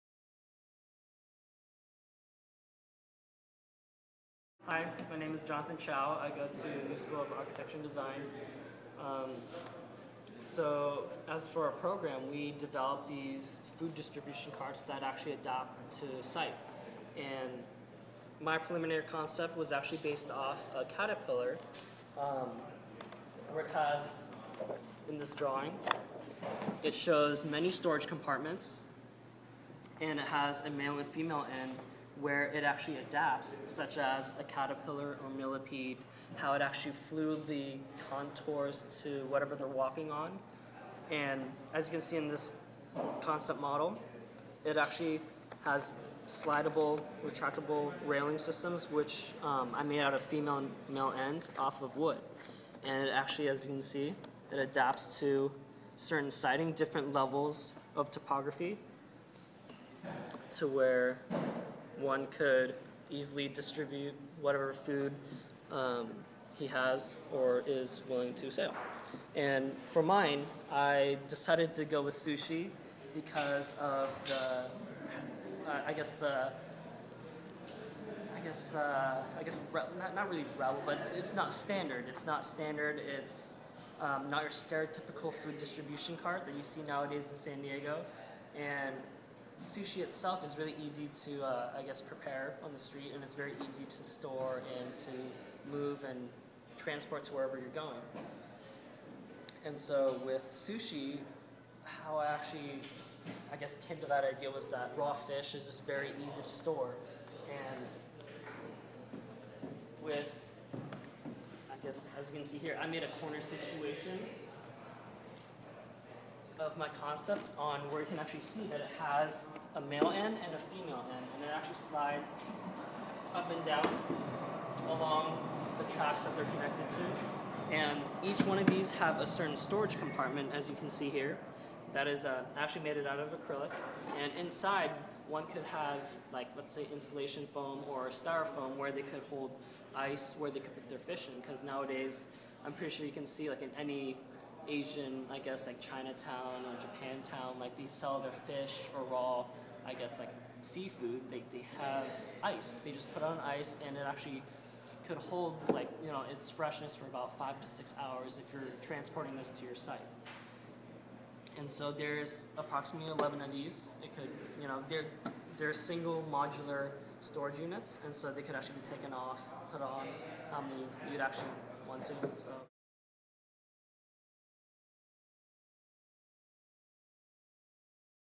NSAD student Jonathan Chau's sushi food cart
NewSchool of Architecture and Design second year student Jonathan Chau created a unique sushi food cart as part of his architecture design studio.
What does a food cart have to do with design? A design studio class at NewSchool of Architecture and Design (NSAD) headed by instructor Hector Perez explored the unusual typology of human-propelled food carts. These are smaller carts that vendors sell food from -- not to be confused with food trucks. The project was done in collaboration with another NSAD design studio lead by NSAD instructor Casey Mahon that explored digital manipulations of surface. The integrated assignments challenged the students (in their second year of studies in NSAD's Bachelor of Architecture program) to explore the interaction possibilities of food carts in a parking lot near San Diego's downtown ball park. The hypothetical scenario -  in which sections of the parking lot could be designated as spaces for the food carts during tailgating events -  encouraged students to evaluate the relationships between sites and projects (the parking lot) and social dynamics (the tailgating scene). Such explorations of function, scale and site form the basis of any architecture or design project, and allows students to develop skills they can use in the future for projects as small as a food cart and as large as a skyscraper.